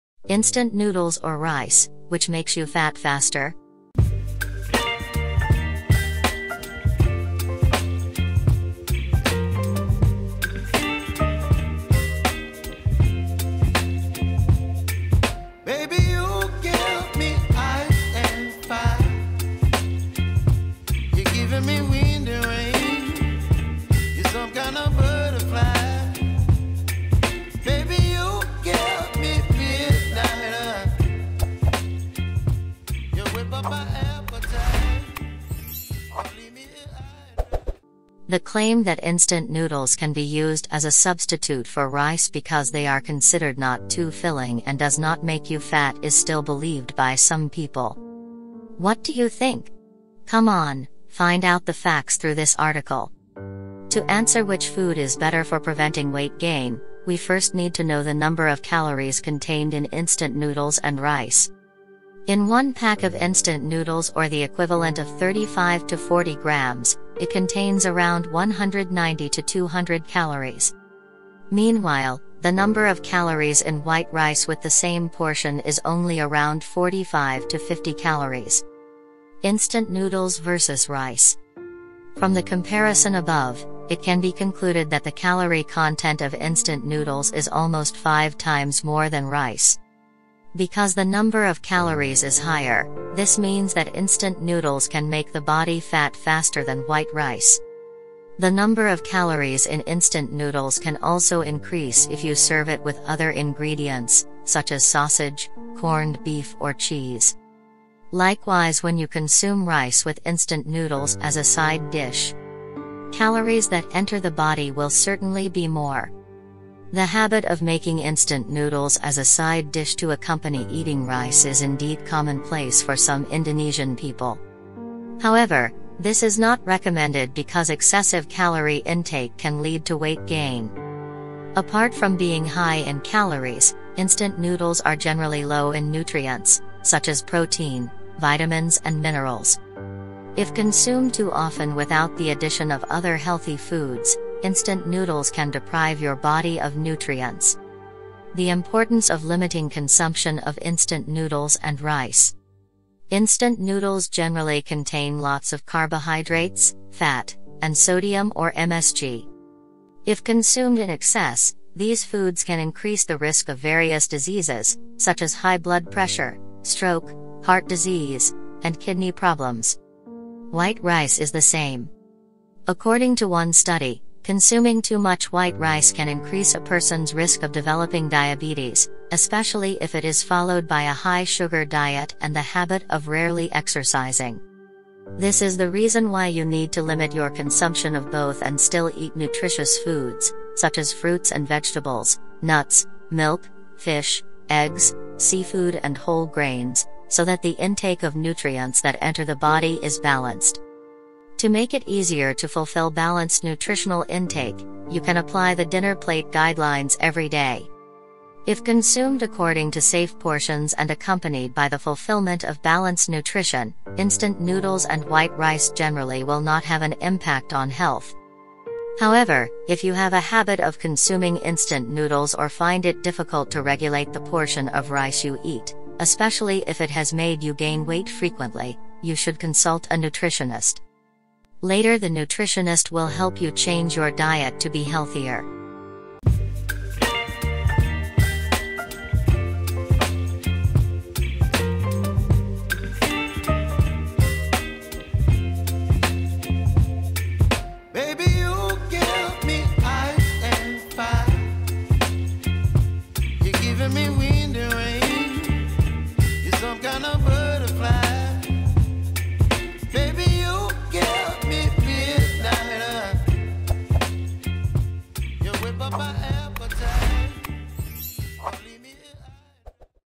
Instant Noodles or Rice, Which Makes You Fat Faster
The claim that instant noodles can be used as a substitute for rice because they are considered not too filling and does not make you fat is still believed by some people. What do you think? Come on, find out the facts through this article.
XHIT Daily.
Tone It Up.
POPSUGAR Fitness.
DOYOUYOGA.com.